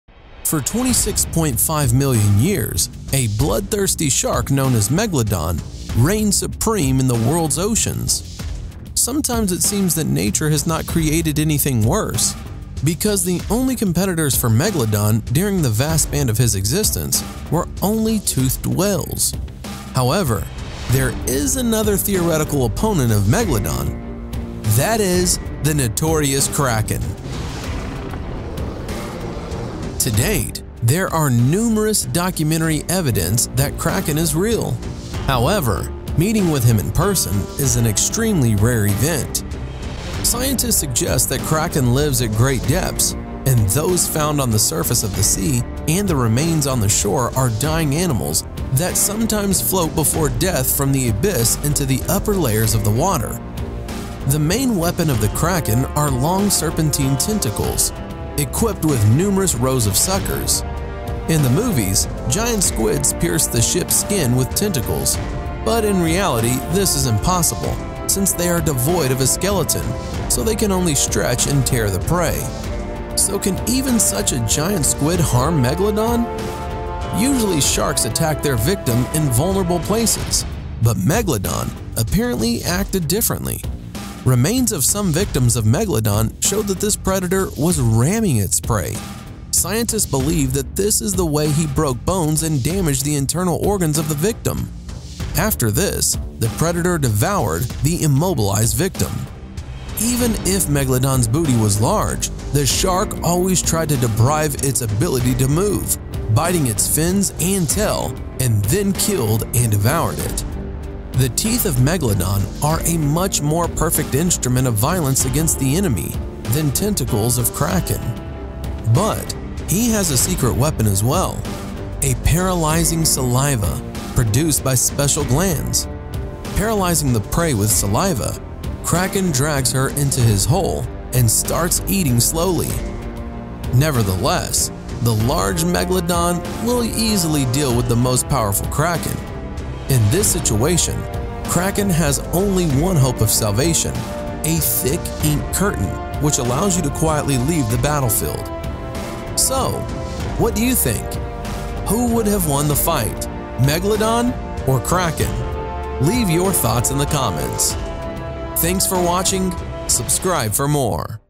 Megalodon VS Kraken. Who would win?
For 26.5 million years, a giant bloodthirsty shark, known as Megalodon, reigned in the world's oceans. Sometimes it seems that nature has not created anything more terrible. The only competitors of Megalodon, during the vast span of his existence, were only toothed whales. Another theoretical opponent of Megalodon is the notorious Kraken. To date, there are numerous documentary evidence that Kraken is a reality. However, meeting a person with him is an extremely rare event. Scientists suggest that Kraken lives at great depth, and those found on the surface of the sea and the remains on the shore are dying animals that sometimes float before death from the abyss into the upper layers of the water. The main weapon of the Kraken are long serpentine tentacles, equipped with numerous rows of suckers. In the movies, giant squids pierce the ship's skin with tentacles, but in reality this is impossible, since they are devoid of a skeleton, so they can only stretch and tear the prey. So can even such a giant squid harm Megalodon? Usually sharks attack their victim in vulnerable places, but Megalodon apparently acted differently. Remains of some victims of Megalodon showed that this predator was ramming its prey. Scientists believe that this way he broke bones and damaged the internal organs of the victim. After this, the predator devoured the immobilized victim. Even if Megalodon's booty was large, the shark always tried to deprive it of its ability to move, biting its fins and tail, and then killed and devoured it. The teeth of Megalodon are a much more perfect instrument of violence against the enemy than the tentacles of Kraken. But he also has a secret weapon - a paralyzing saliva, produced by special glands. Paralyzing the prey with saliva, Kraken drags her into his hole and starts eating slowly. Nevertheless, the large Megalodon will easily deal with the most powerful Kraken. In this situation, Kraken has only one hope of salvation - a thick ink curtain, which allows you to quietly leave the battlefield.
So, what do you think, who would have won the fight, Megalodon or Kraken?